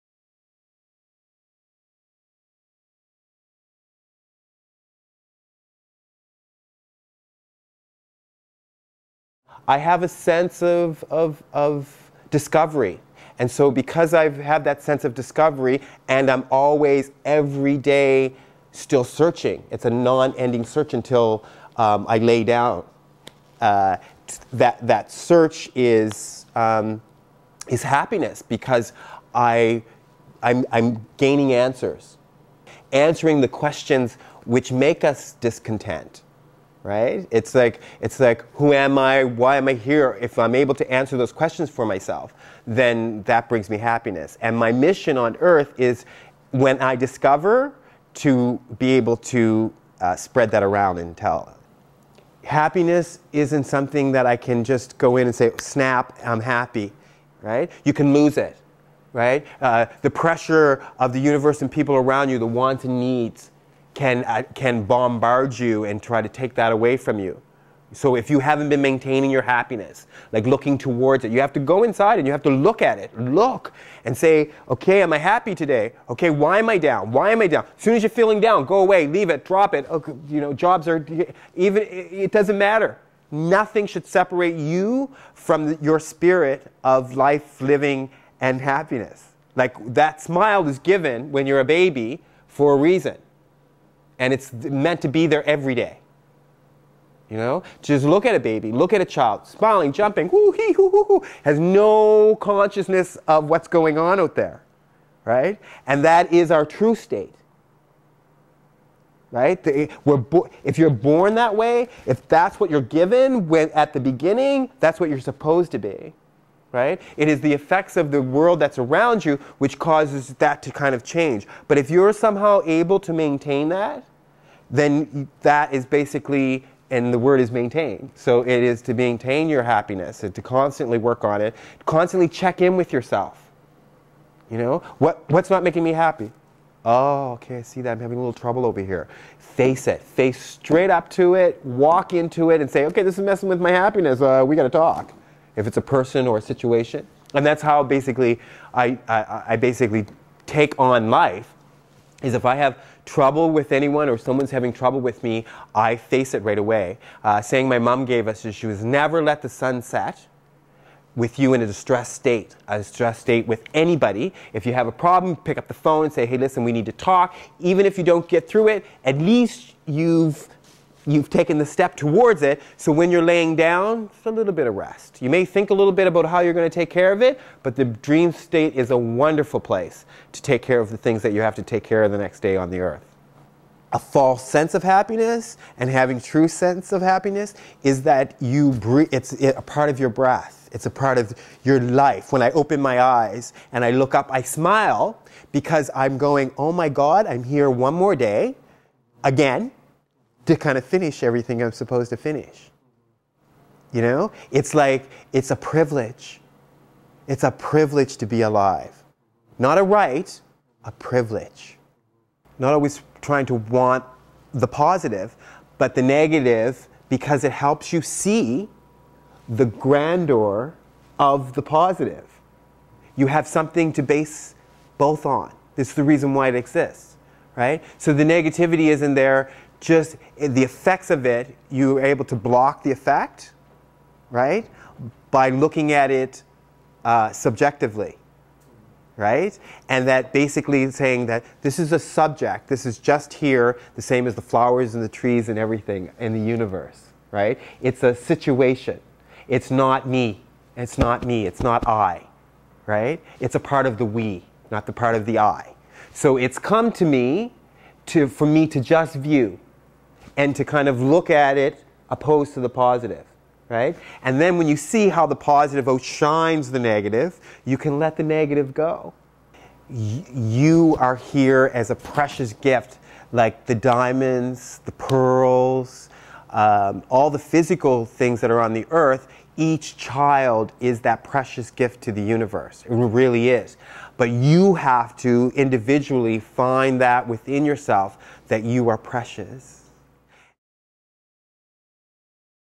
Happiness-Seekers positive thinking by Tyrone Bruce
My mission is to be happy all the time. You can lose it, you have to go inside and look at at it. Nothing should separate you from your happiness.